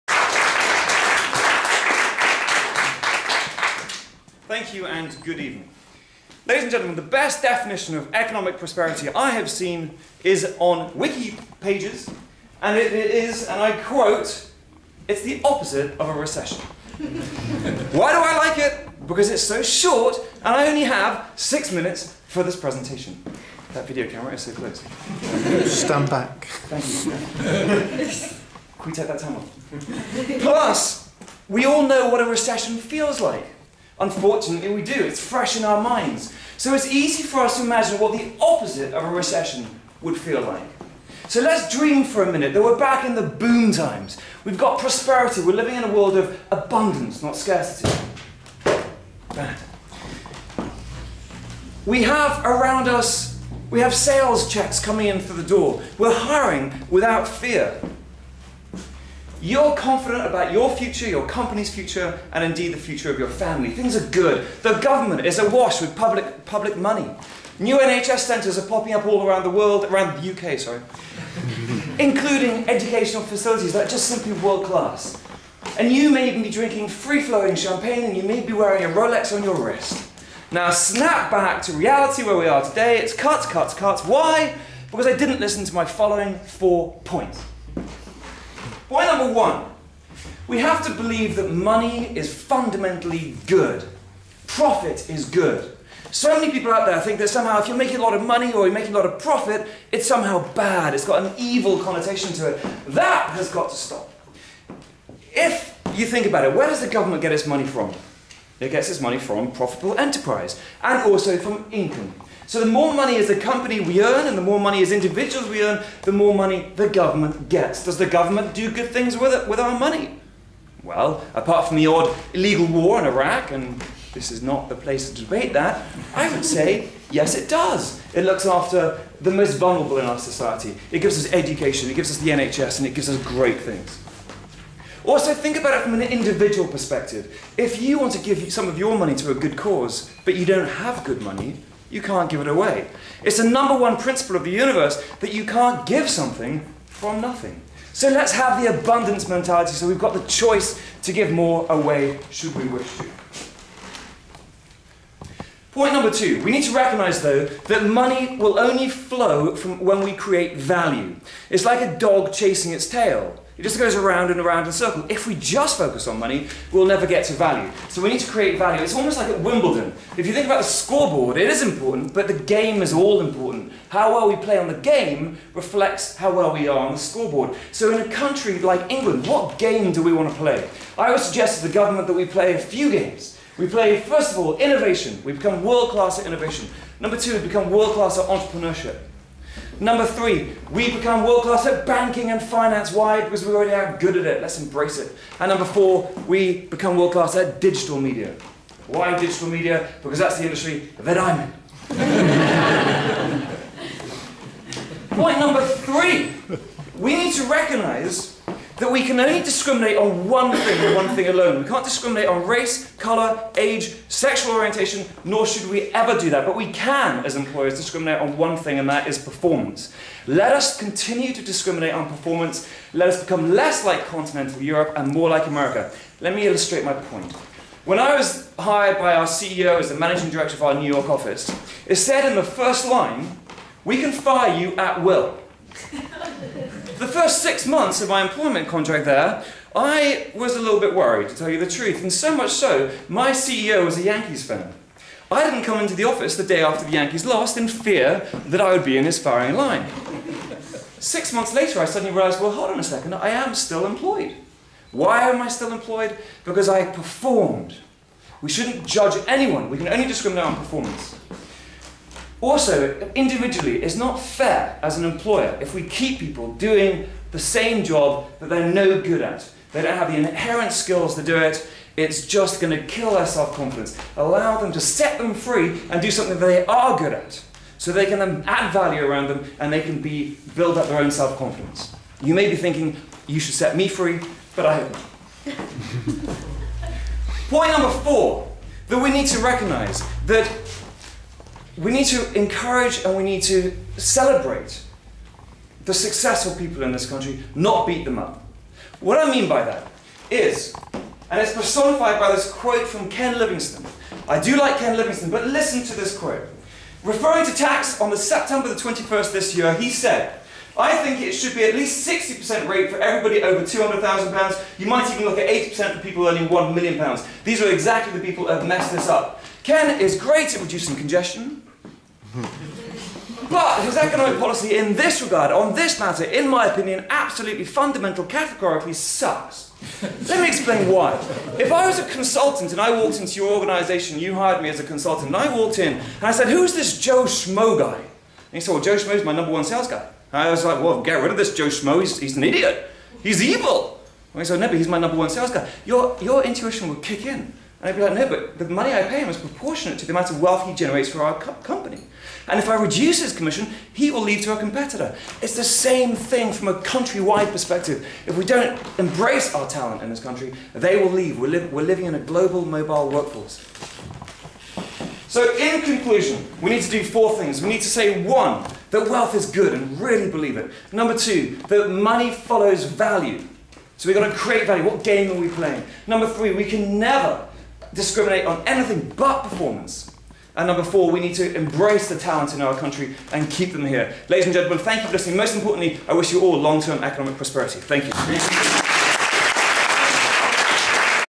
Anthony Gell at the College of Public Speaking Corporate Challenge 2010
Anthony Gell's runner-up speech at the College of Public Speaking Corporate Challenge at the English-Speaking Union, Mayfair on the 25th of October 2010.